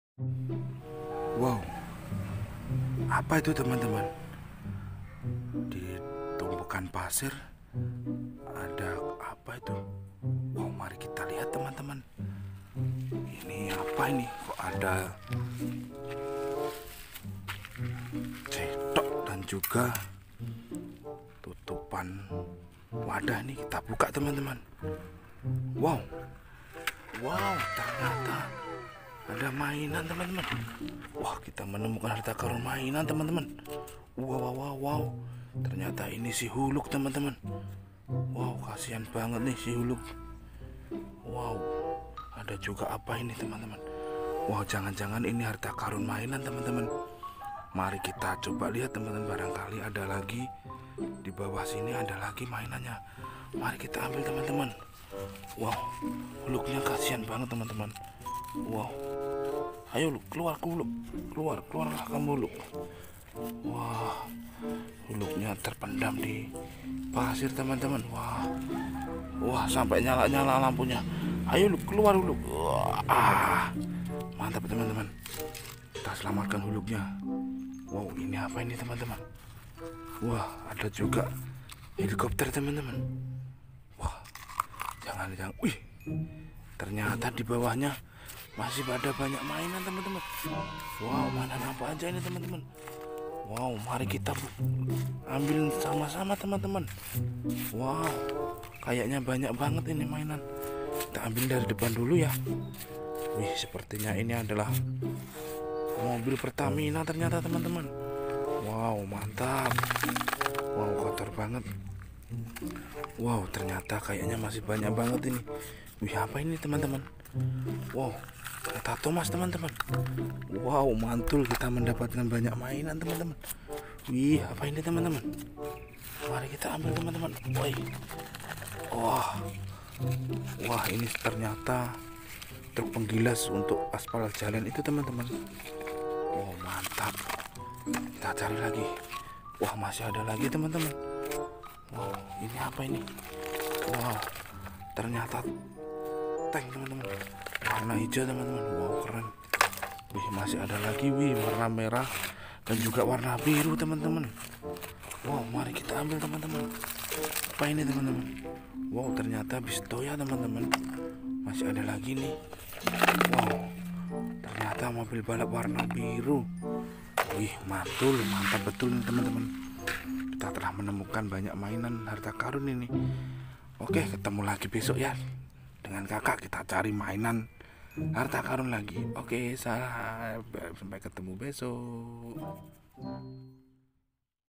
Mencari harta Karun mainan Hulk, helikopter,mobil balap,bus tayo,tank
Mencari harta Karun mainan di tumpukan pasir